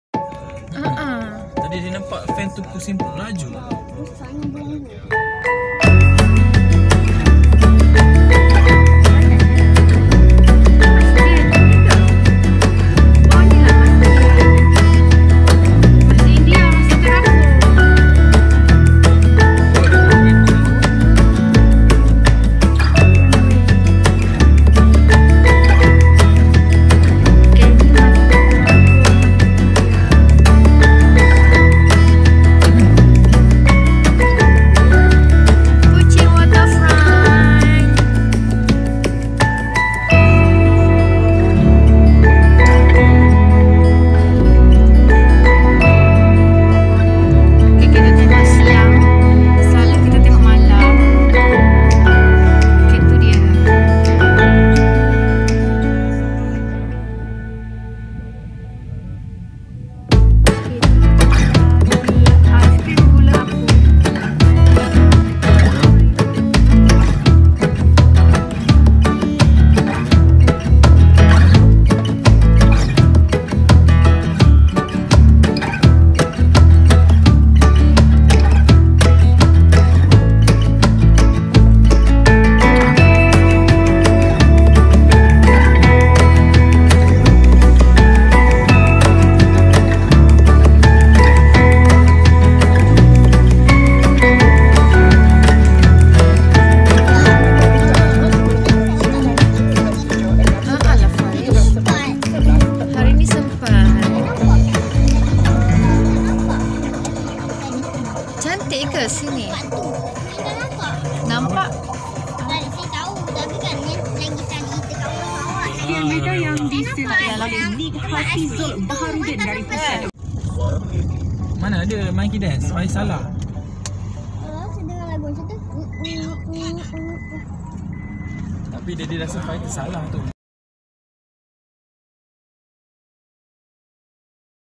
Kucing Waterfront
Sampai Kucing mesti jalan2 sini. Ada Masjid Terapung untuk Solat, ade Sarawak Cruise, ada gerai2 makan, gerai Aiskrim Gula Apong, boleh jalan2 sepanjang deretan kedai untuk tengok sourvenior dan kek lapis juga. Malam boleh menikmati lampu2 yang cantik